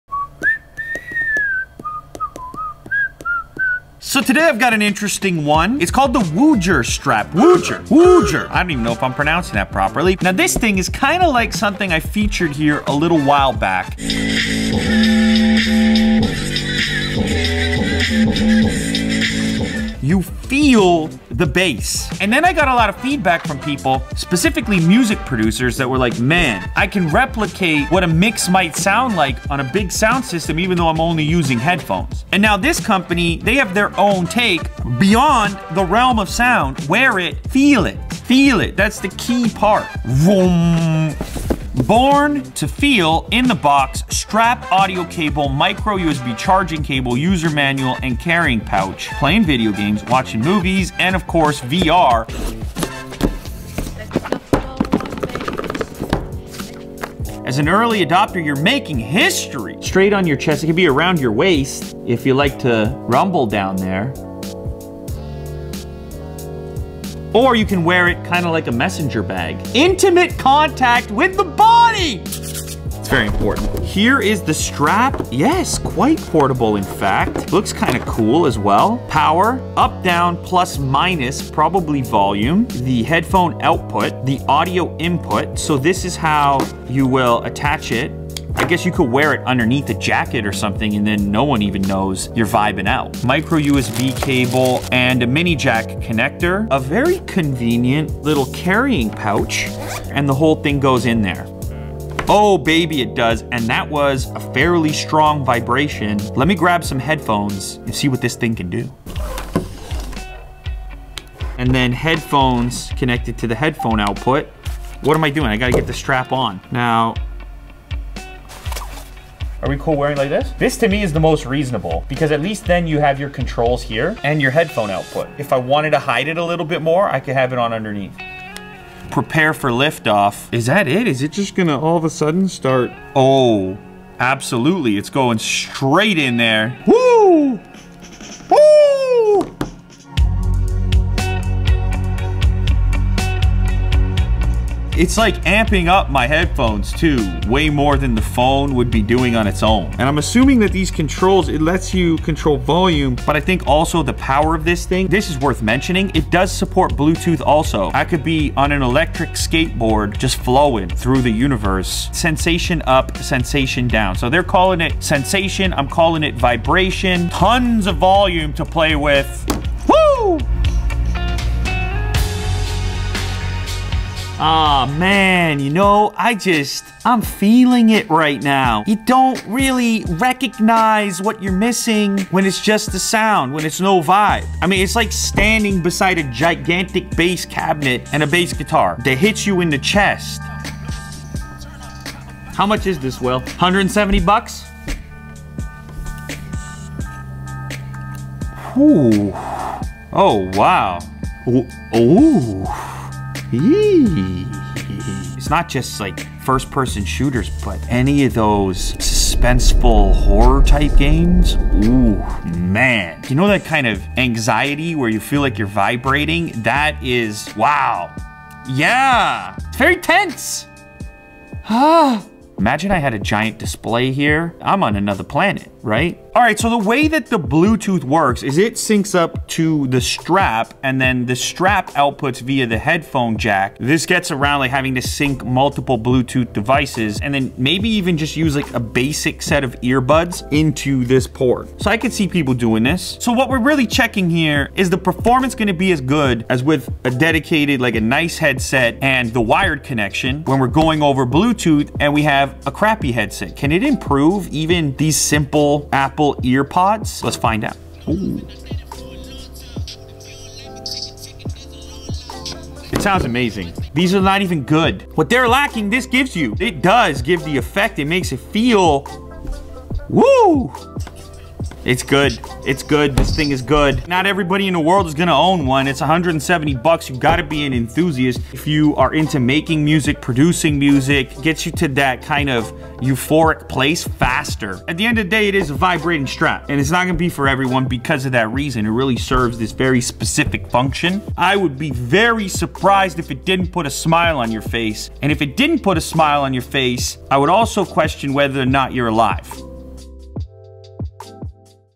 You Wish You Got THIS For Christmas...
The Woojer strap adds some serious oomph to your audio. Whether it's music, video games or movies. This thing seriously enhances the audio experience.